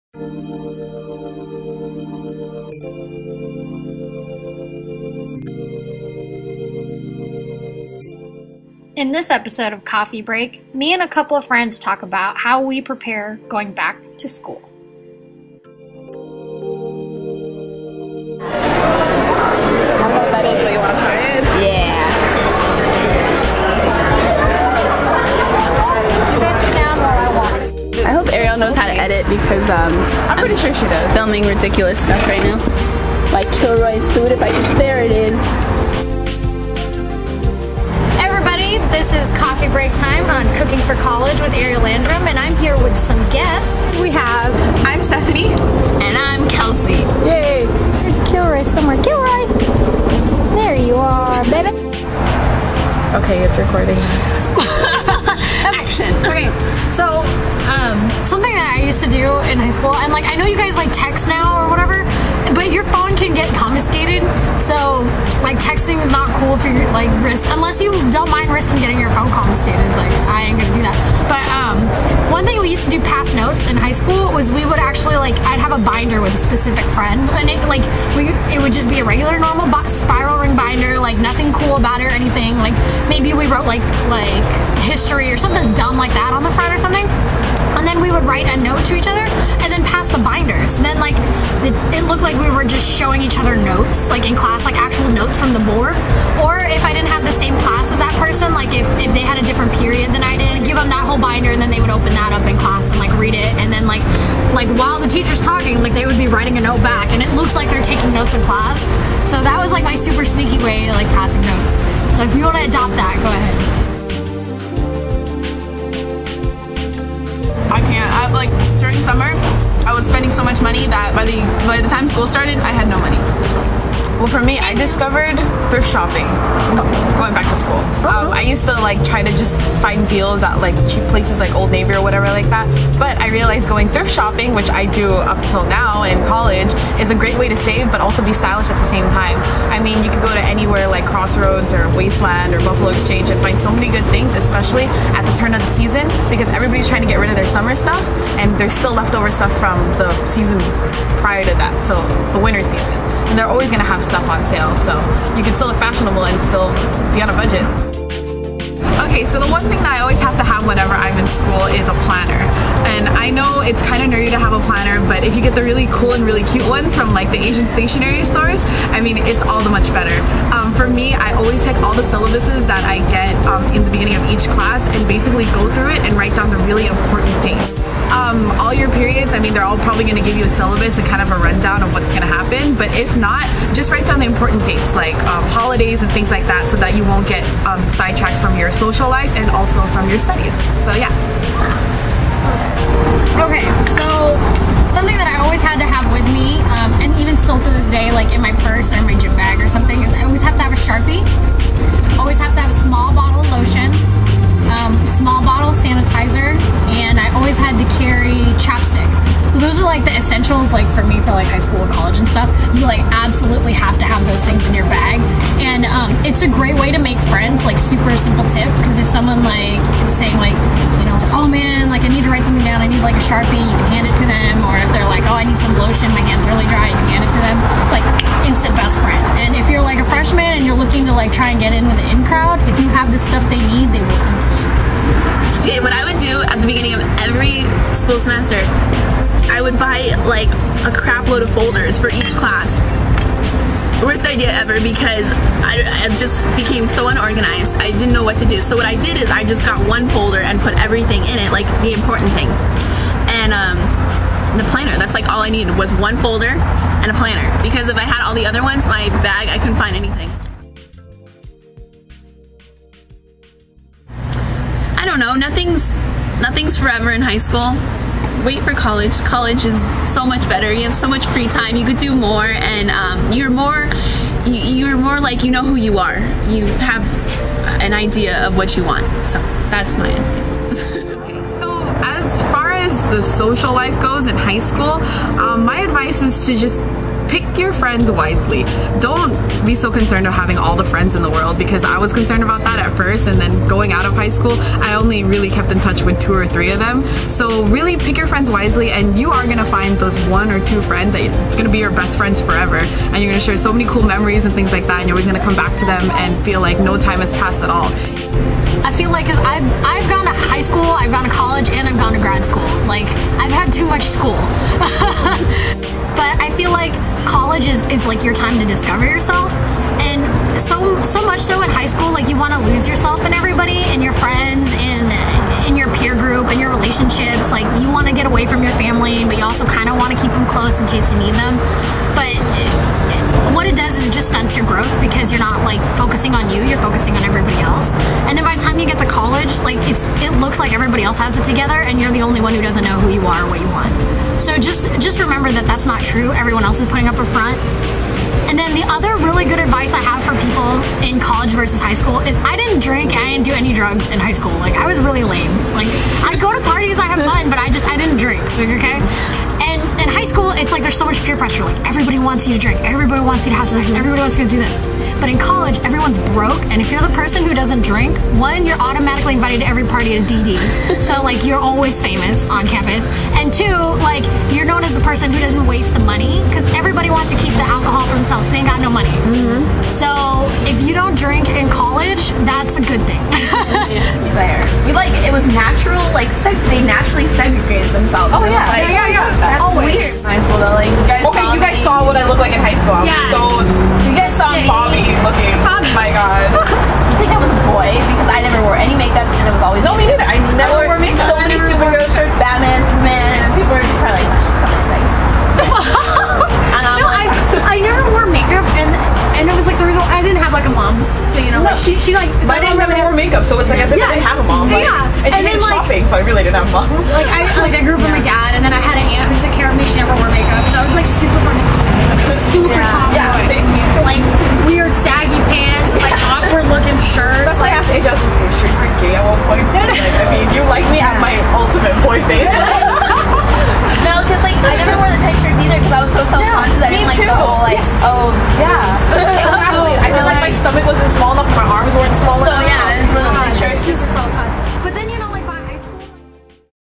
Coffee Break: Back To School
READ ME :)

The Coffee Break:
A time to answer viewers questions, share upcoming projects on my YouTube channel, and share tips and tricks. Enjoy!

Back To School:
In this vlog, I share some ideas to help you prepare going back to school! Guests hosts, Stef and Kels, give their tips and tricks to surviving high school and college life, as well.